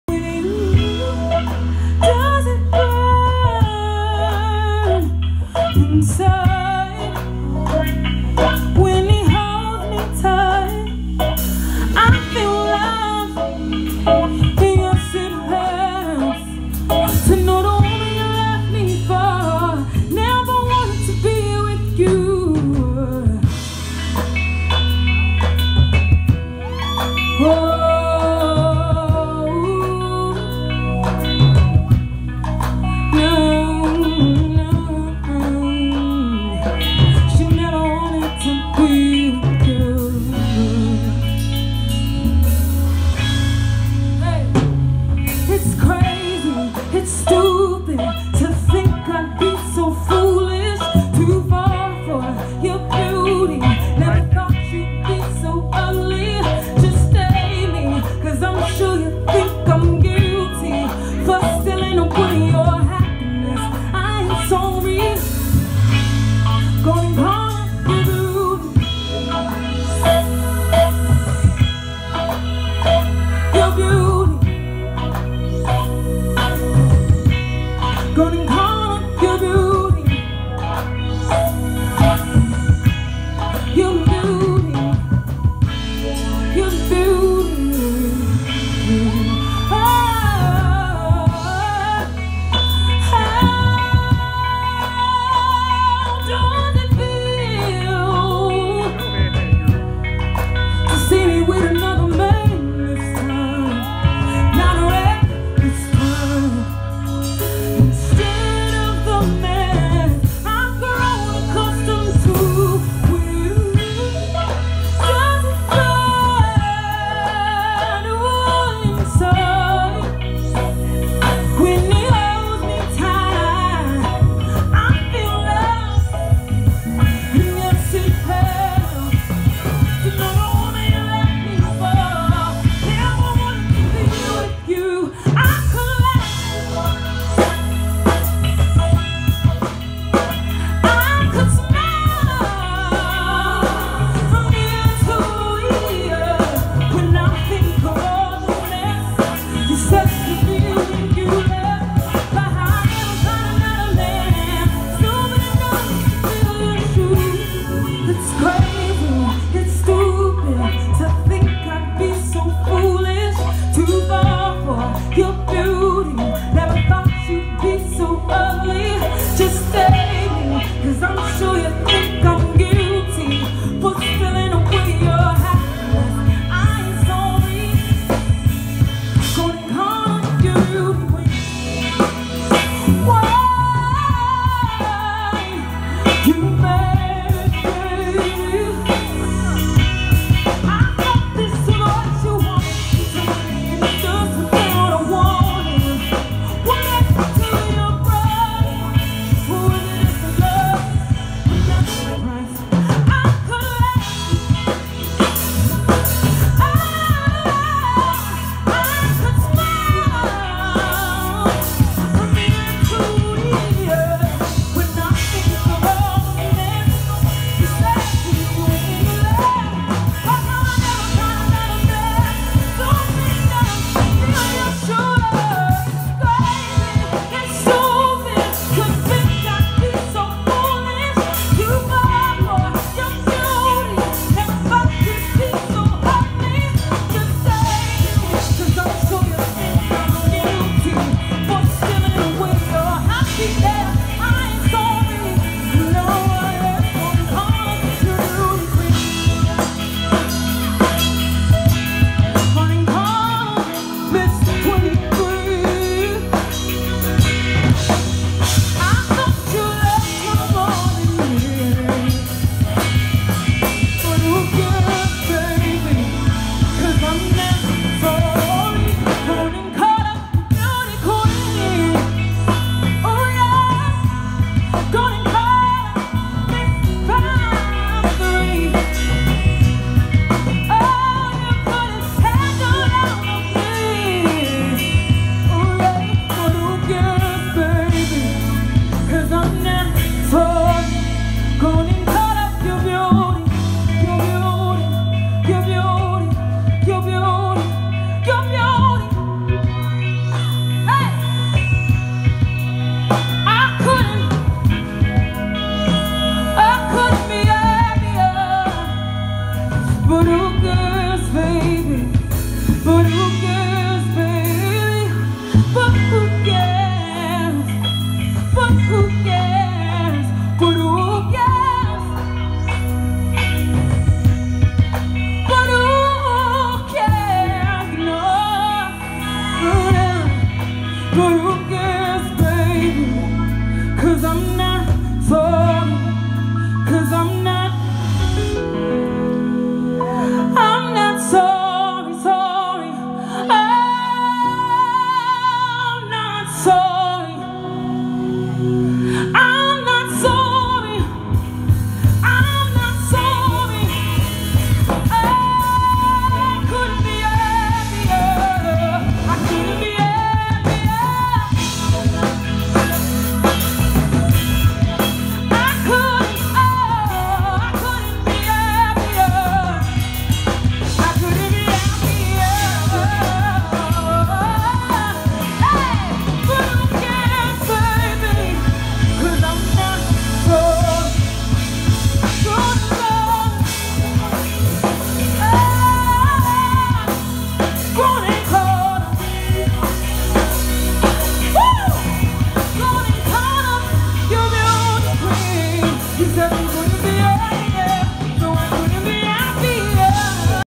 Gabrielle NICOLE, MaKynLee Elise's Cousin performing at Rock & Roll Hall of Fame
Gabrielle Nicole - Topic Makynlee Elise - Topic MaKynLee Elise's Cousin, Gabrielle NICOLE performing at Rock and Roll Hall of Fame.